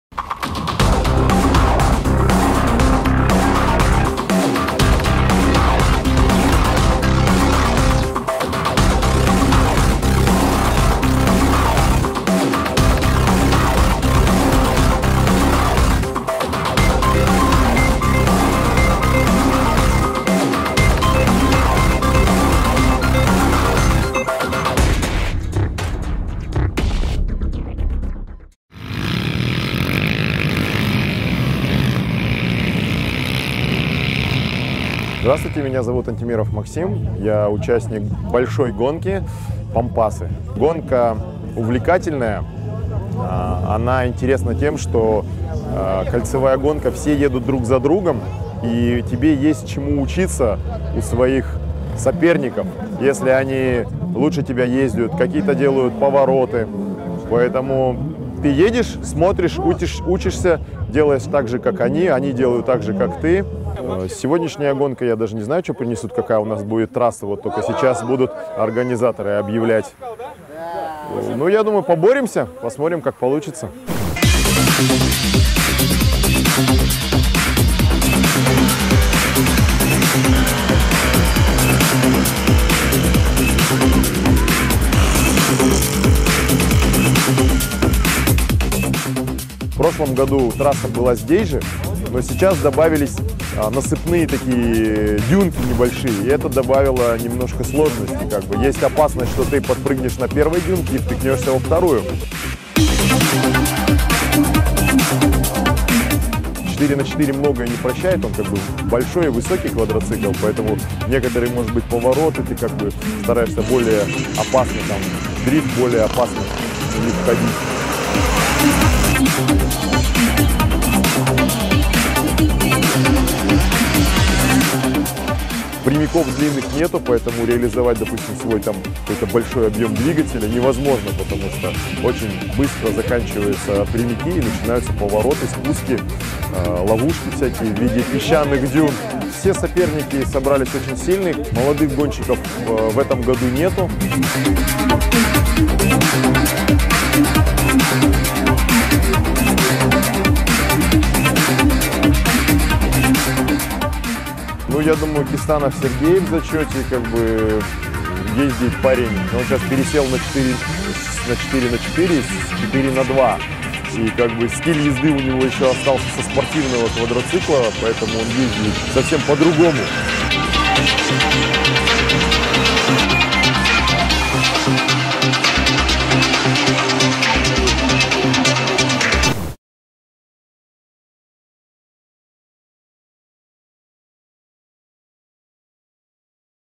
Эндуро-спринт "Пампасы" 2014 часть5/Pampas 2014 Atv Utilit pt 5
Осенний Кубок "Большая Гонка" 11-12 октября, Капчагай